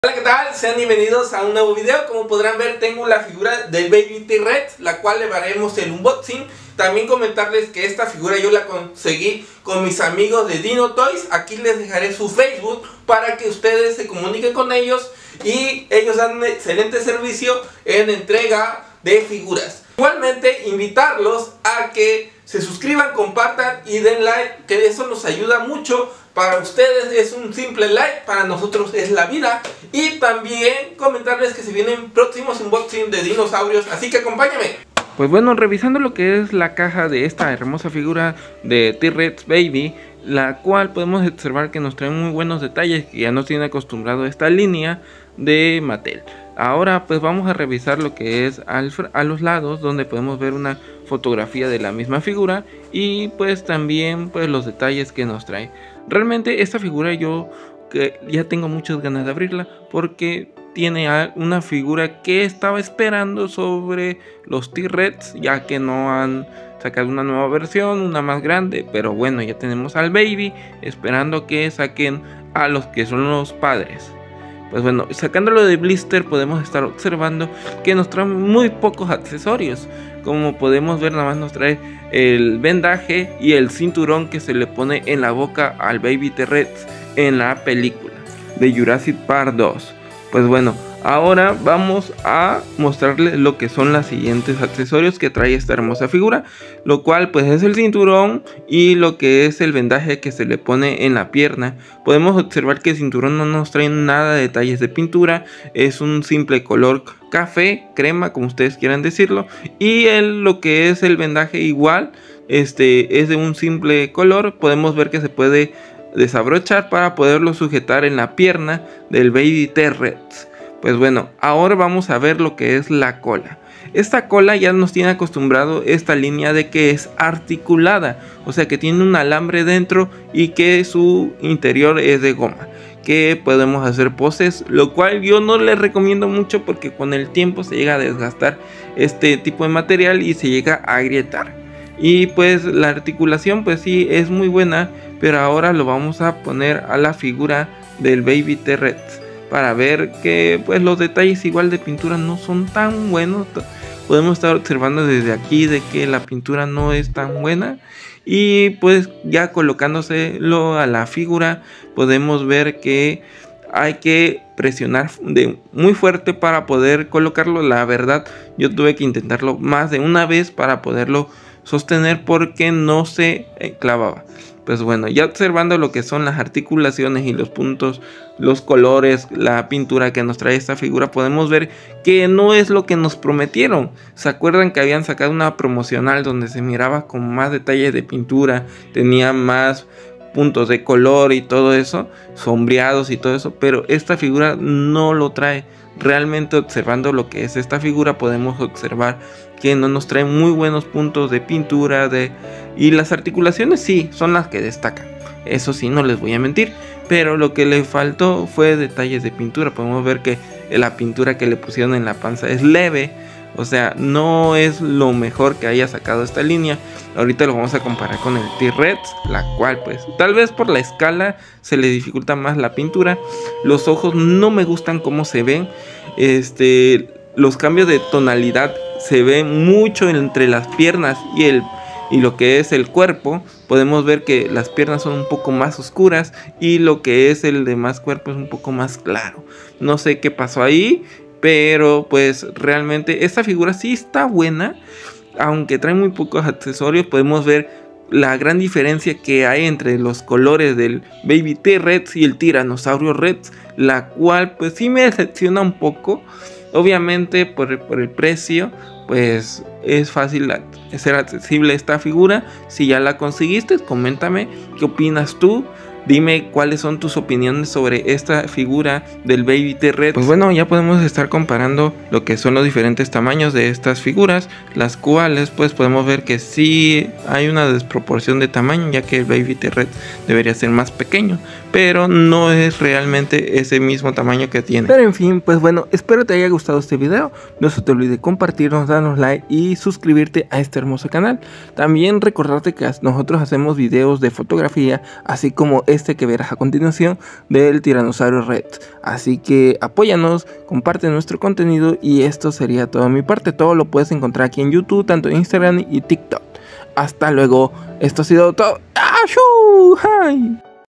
Sera la figura más FEA de Jurassic Park Hammond Collection Tyrannosaurus Rex (Juvenile)
🔴 AYUDANOS COMPARTIENDO EL CONTENIDO

TIENDA: DINO TOYS TIENDA DE FIGURAS

etiquétanos en tus fotografías en instagram